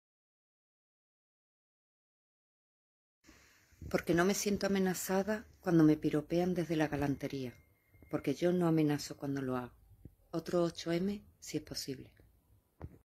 EL 8-M ESTÁ USURPADO POR FEMINISMO DEL ODIO AL HOMBRE. LAS MUJERES SE HARTAN DE LAS FEMINISTAS
Y las mujeres no feministas quieren construir un 8-M que muestra que son compañeras de viaje de los hombres